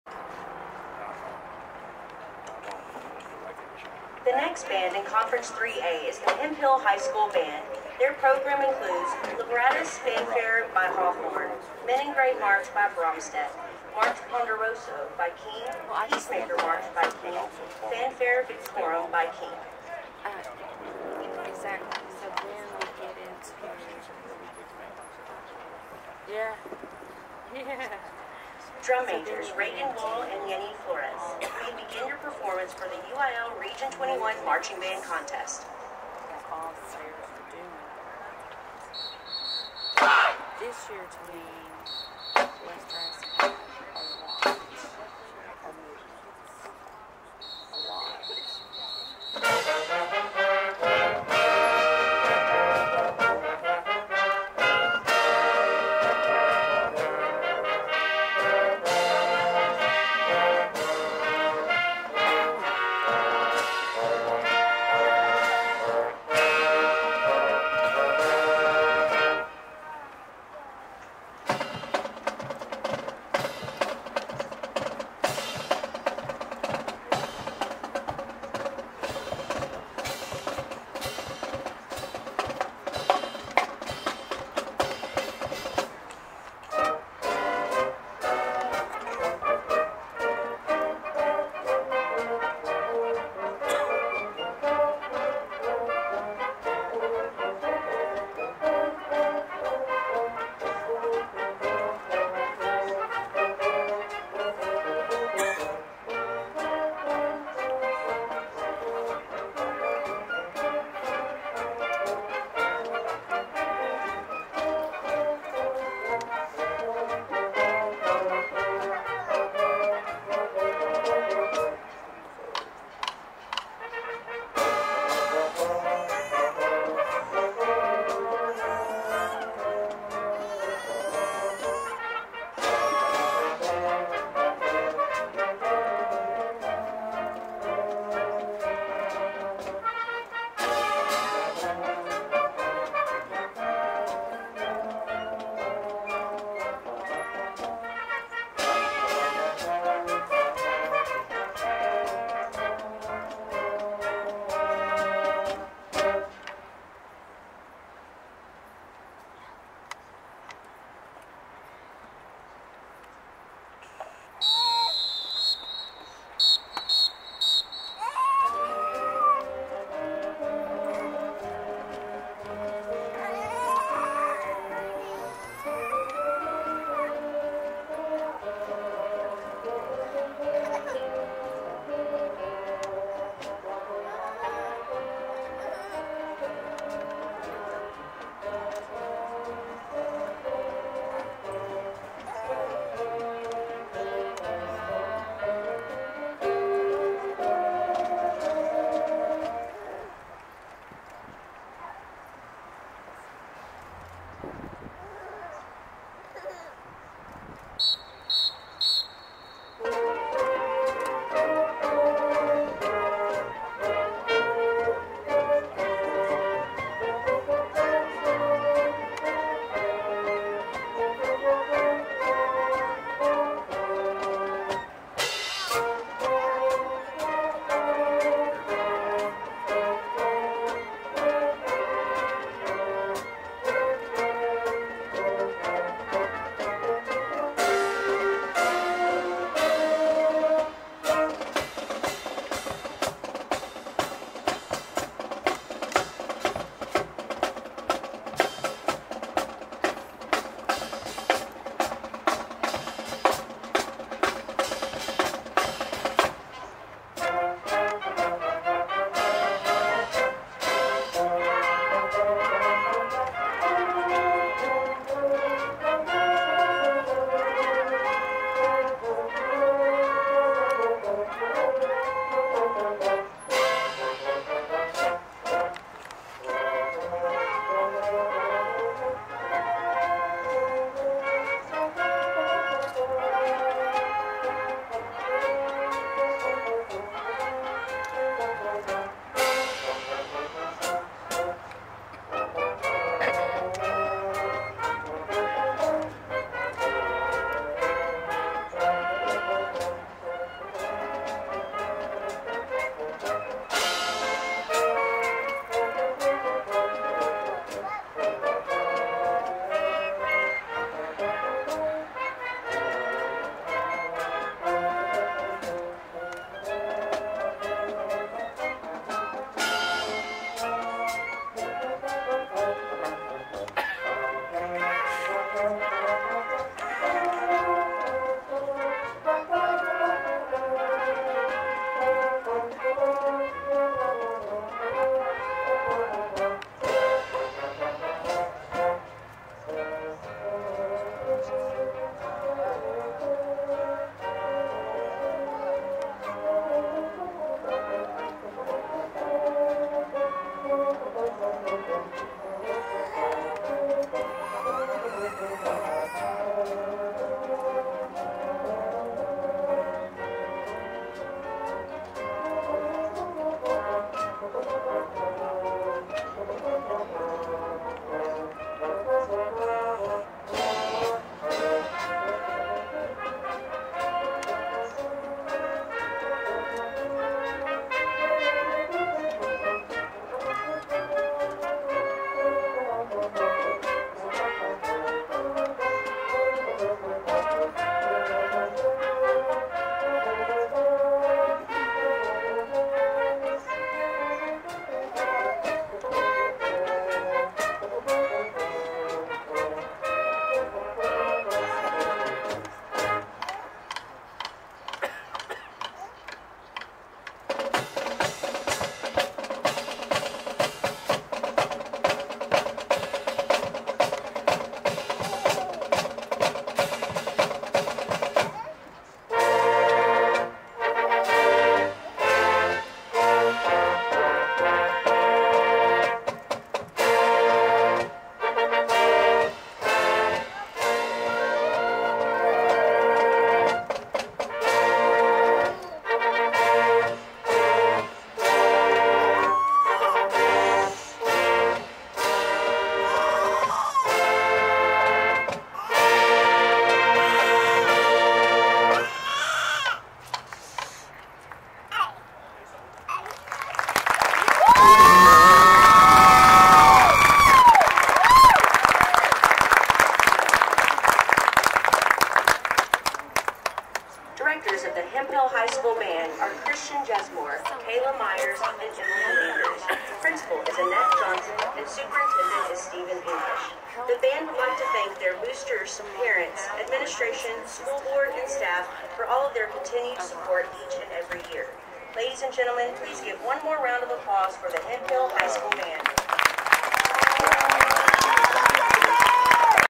Hemphill HS Marching Contest 2022
The Hemphill band performed at the Region 21 Marching Contest at the Longview Lobo stadium on October 15.

Class 3A
Division: I